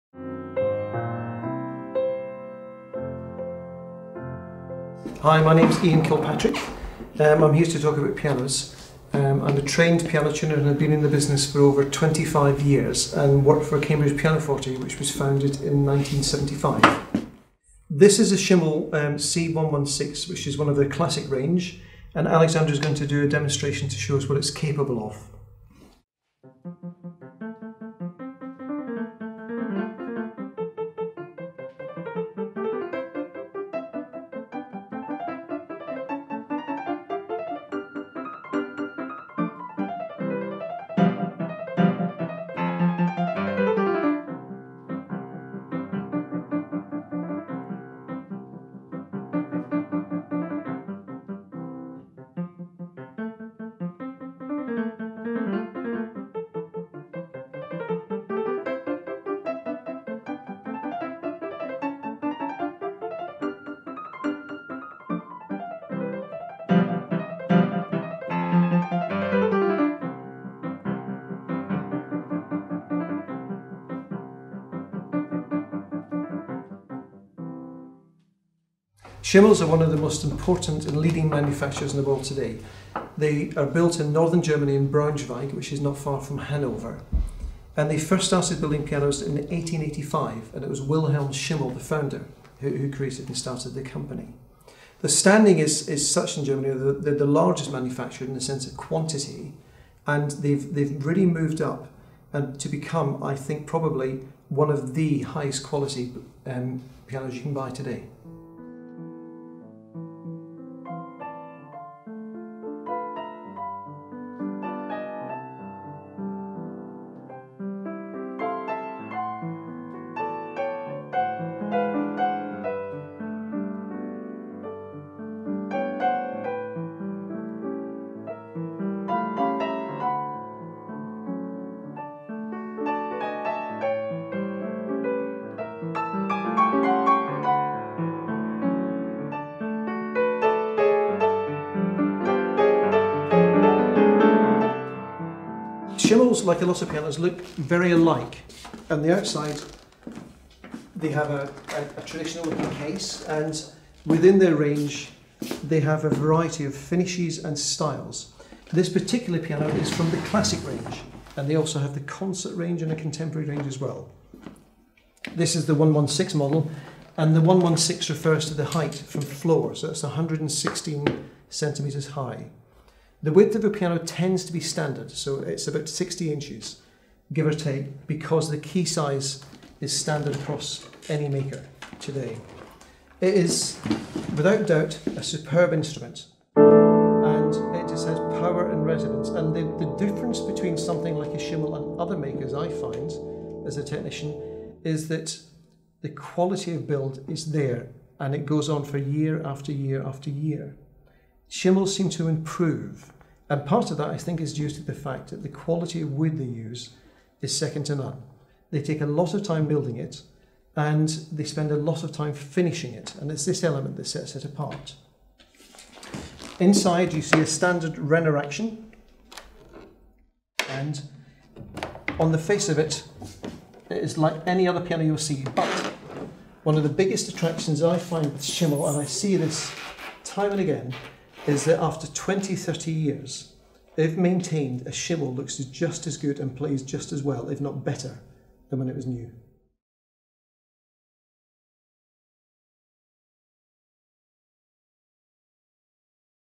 Schimmel C116 Upright Piano
This is Schimmel’s most popular upright piano; it has been available for over a decade and its popularity remains unbroken.

What makes this Piano the best-seller of all Schimmel models? Perhaps because this instrument possesses the ideal combination of reliability, playability, tonal quality and elegance.